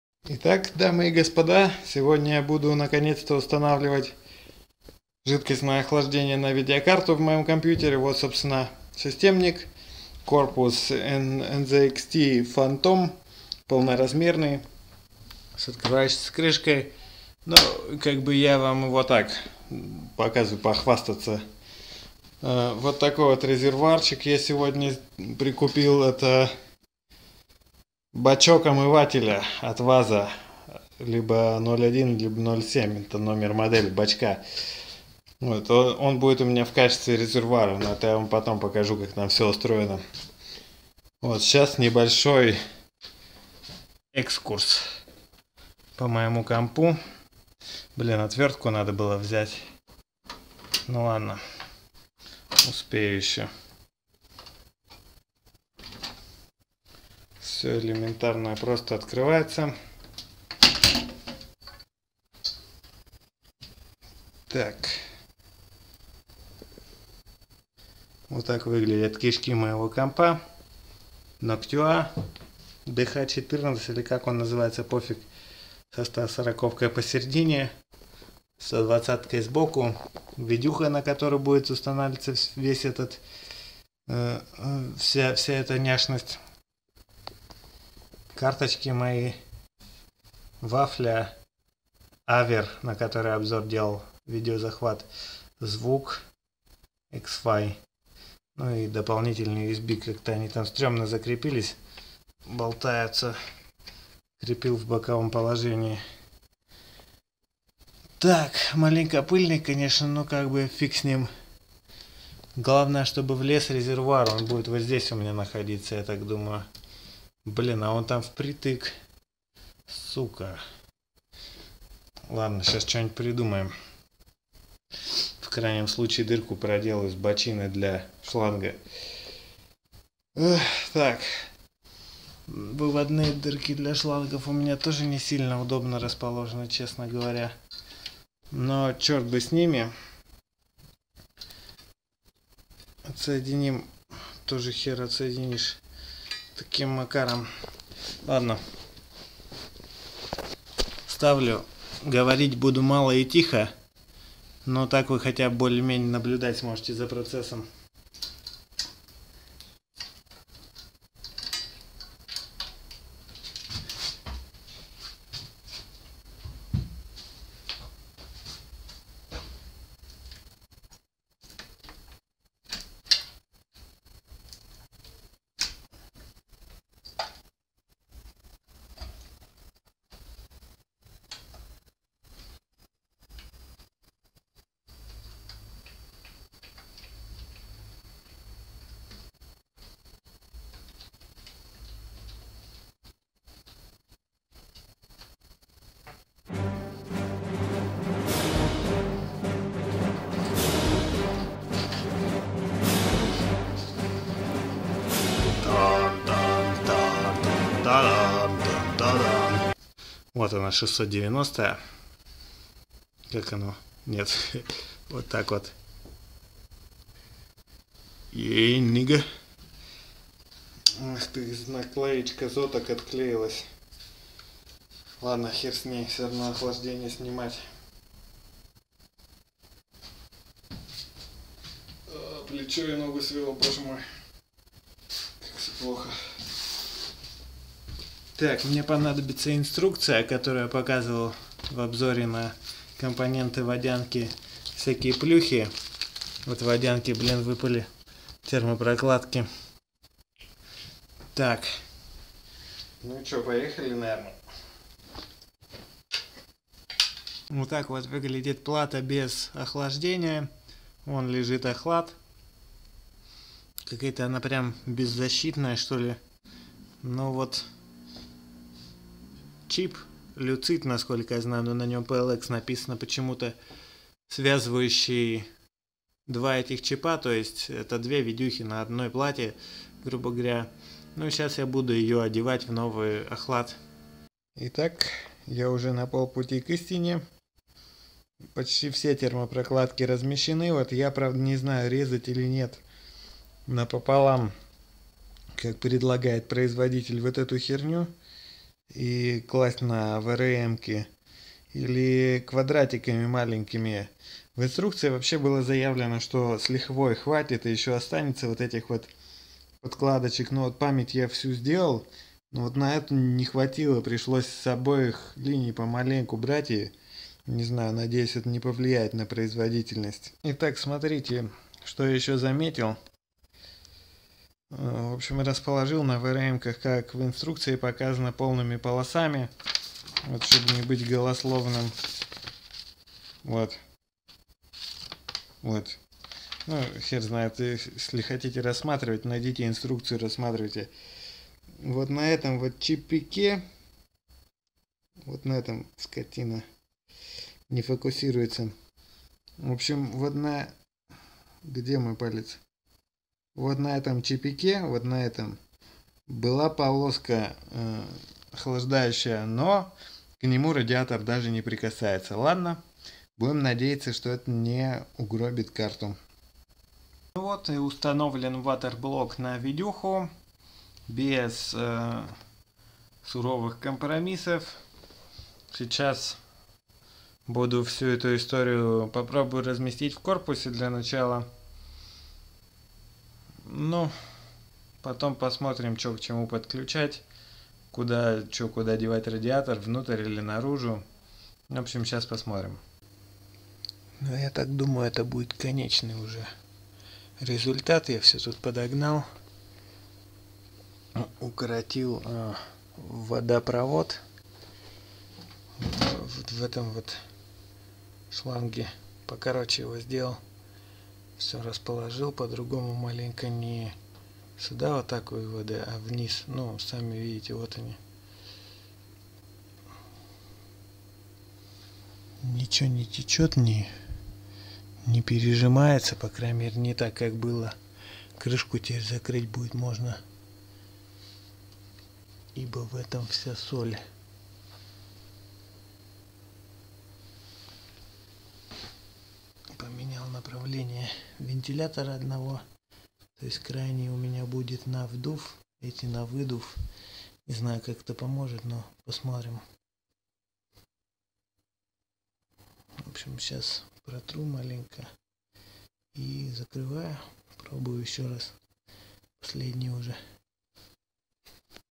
Watercooling for video: Assembling | Сборка СВО для видеокарты
Наконец-то собираю полностью СВО для GF-GTX690.